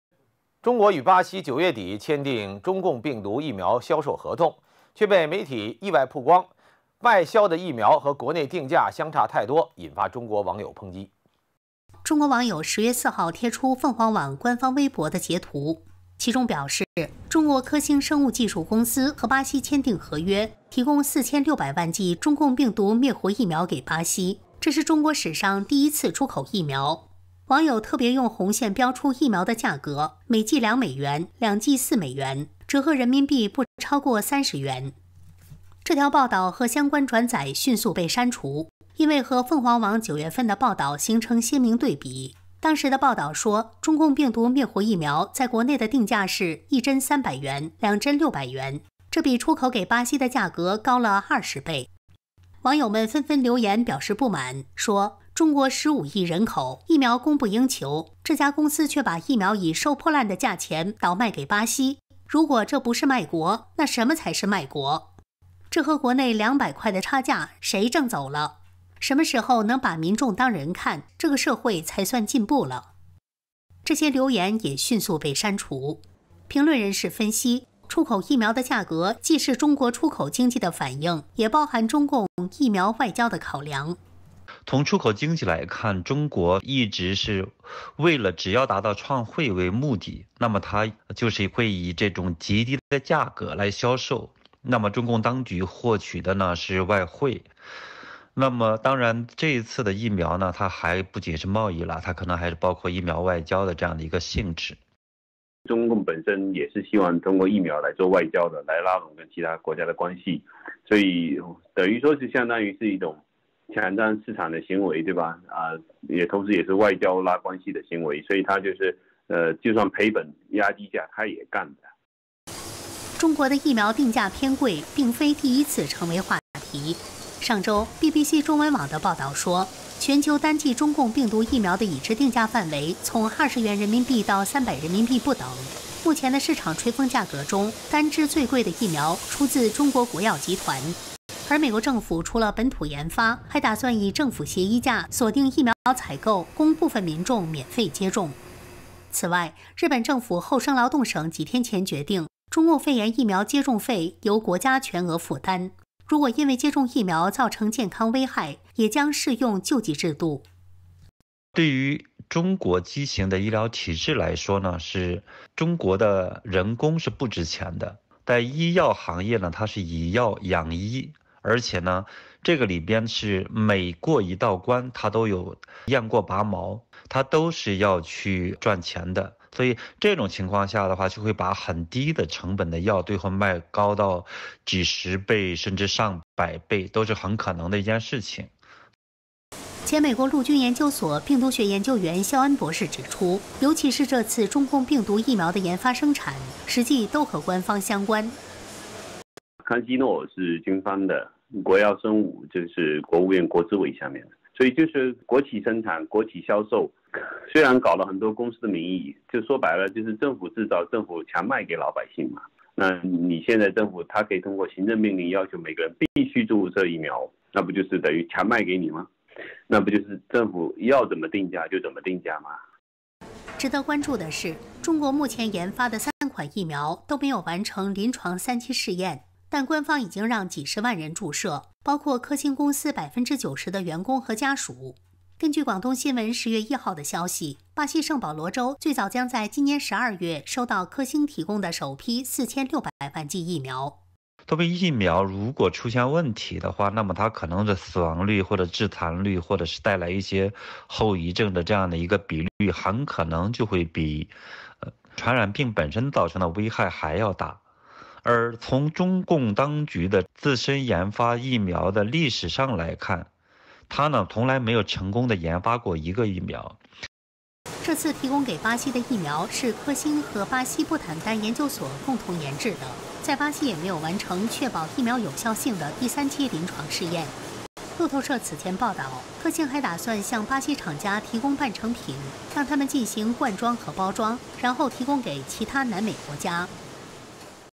国外便宜国内贵？中国疫苗价格引抨击 720P
文章标题：【禁闻】国外便宜国内贵？中国疫苗价格引抨击

【新唐人北京时间2020年10月09日讯】中国与巴西9月底签订中共病毒疫苗销售合同，却被媒体意外曝光，外销的疫苗和国内定价相差太多，引发中国网友抨击。

中国网友10月4号贴出凤凰网官方微博的截图，其中表示，中国科兴生物技术公司和巴西签订合约，提供4,600万剂中共病毒灭活疫苗给巴西。这是中国史上第一次出口疫苗。网友特别用红线标出疫苗的价格“每剂2美元，2剂4美元”，折合成人民币不超过30元。

这条报导和相关转载迅速被删除。因为和凤凰网9月份的报导形成鲜明对比，当时的报导说，中共病毒灭活疫苗在国内的定价是“一针300元、两针600元”，这比出口给巴西的价格高了20倍。

网友们纷纷留言表示不满说，“中国十五亿人口，疫苗供不应求，这家公司却把疫苗以收破烂的价钱倒卖给巴西，如果这不是卖国，那什么才是卖国？！”“这和国内200块的差价，谁挣走了！”“什么时候能把民众当人看，这个社会才算进步了。”

这些留言也迅速被删除。

评论人士分析，出口疫苗的价格既是中国出口经济的反映，也包含中共“疫苗外交”的考量。

旅美政经人士秦鹏：“从出口经济来看，中国一直是为了只要达到创汇为目的，它就是会以这种极低的价格来销售， 中共当局获取的是外汇。那么当然这一次的疫苗它还不仅是贸易，它可能还是包括疫苗外交的这样的一个性质。”

前美国陆军研究所病毒学研究员萧恩博士：“中共本身也是希望通过疫苗来做外交，来拉拢和其他国家的关系。所以等于说是一种抢占市场的行为，同时也是外交拉关系的行为。所以它就算赔本压低价它也干。”

中国的疫苗定价偏贵并非第一次成为话题。

上周BBC中文网的报导说，全球单剂中共病毒疫苗的已知定价范围从20元人民币（3美元）到300人民币（44美元）不等。目前的市场吹风价格中，单支最贵的疫苗出自中国国药集团。而美国政府除了本土研发，还打算以“政府协议价”锁定疫苗采购，供部分民众免费接种。

此外日本政府厚生劳动省几天前决定，中共肺炎疫苗接种费用由国家全额负担，如果因为接种疫苗造成健康危害，也将适用救济制度。

秦鹏：“对于中国畸形的医疗体制来说，中国的人工是不值钱的。在医药行业他是以药养医。而且这个里边每过一道关，他都有雁过拔毛，他都是要去赚钱的。所以这种情况下的话，就会把很低的成本的药最后卖高到几十倍甚至上百倍，都是很可能的一件事情。”

前美国陆军研究所病毒学研究员萧恩博士指出，尤其是这次中共病毒疫苗的研发生产，实际都和官方相关。

萧恩博士：“康希诺是军方的，国药生物就是国务院国资委下面的。所以就是国企生产国企销售，虽然搞了很多公司名义，说白了就是政府制造，政府强卖给老百姓，现在政府它可以通过行政命令要求每个人必须注射疫苗，那不就是等于强卖给你吗？那不是政府要怎么定价就怎么定价吗？”

值得关注的是，中国目前研发的三款疫苗都没有完成临床三期试验，但官方已经让几十万人注射，包括科兴公司90%的员工和家属。根据广东新闻10月1号消息，巴西圣保罗州最早在今年12月收到科兴提供的首批4600万剂疫苗。

秦鹏：“作为疫苗如果出现问题的话，那么它可能的死亡率或者致残率，或者是带来一些后遗症的这样的一个比率，很可能就会比传染病本身造成的危害还要大。而从中共当局的自身研发疫苗的历史上来看，它从来没有成功的研发过一个疫苗。”

这次提供给巴西的疫苗是科兴和巴西Butantan研究所共同研制的，在巴西也没有完成确保疫苗有效性的第3期临床试验。路透社此前报导，科兴还打算向巴西厂家提供半成品，让他们进行灌装和包装，然后提供给其他南美国家。

采访/常春 编辑/尚燕 后制/王明宇

然后人数。

大纪元

2005年1月12日